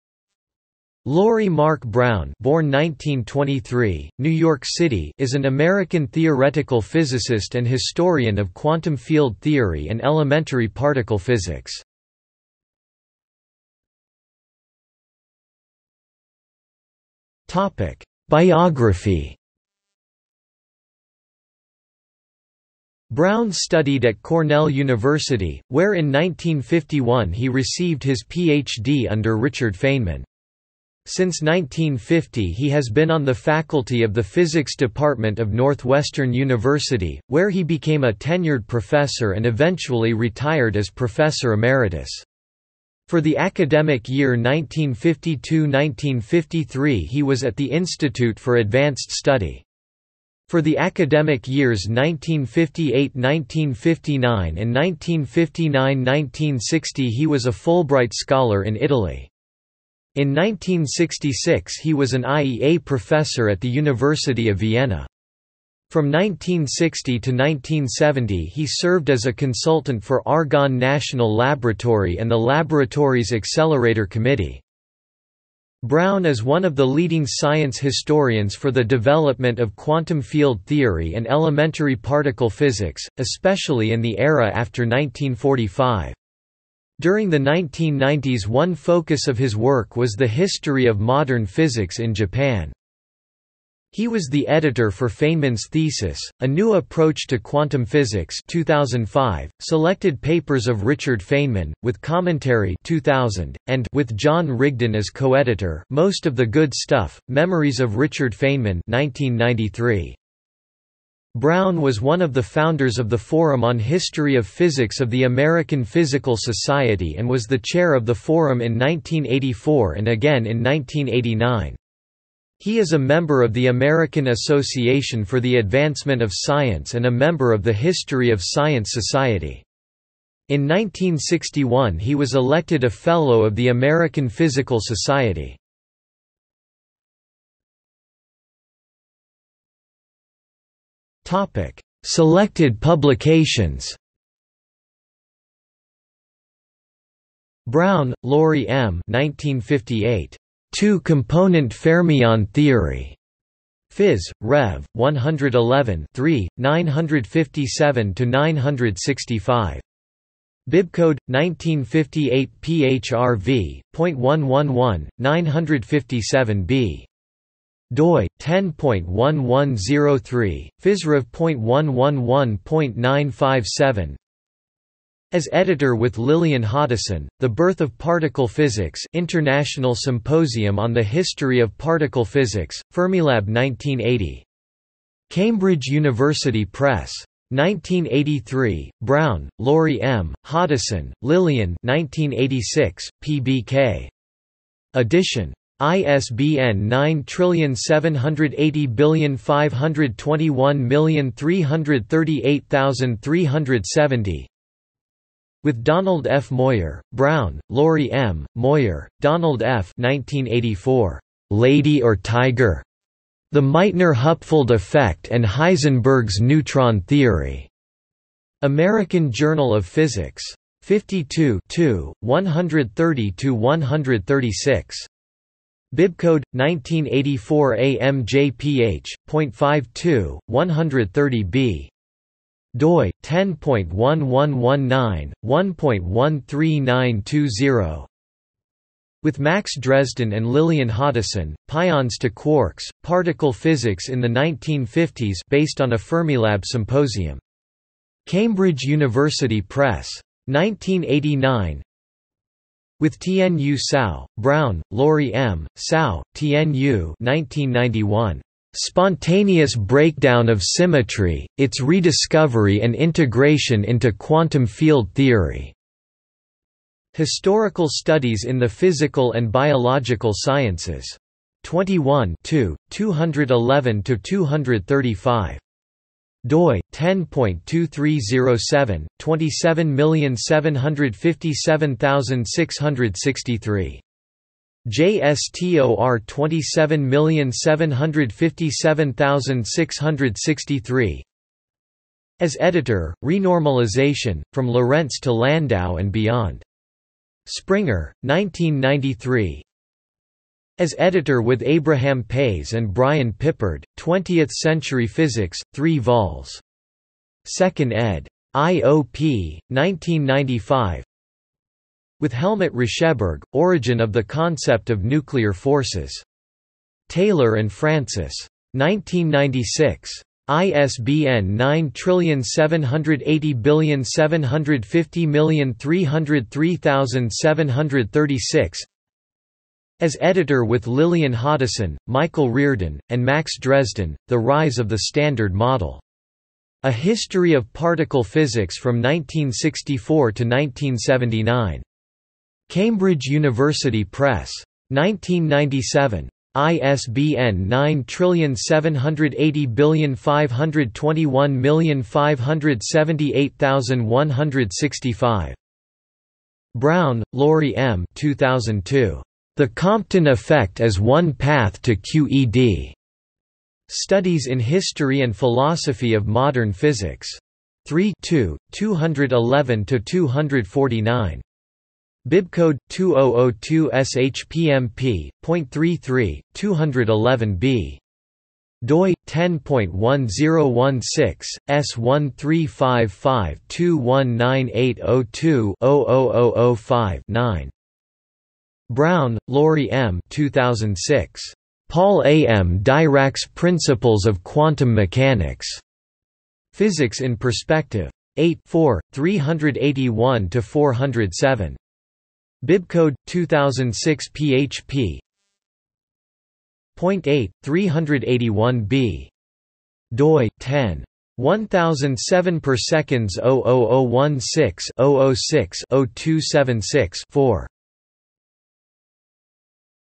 Laurie Brown (physicist) | Wikipedia audio article
00:00:13 Biography
00:02:17 Selected publications

- Socrates

SUMMARY
Laurie Mark Brown (born 1923, New York City) is an American theoretical physicist and historian of quantum field theory and elementary particle physics.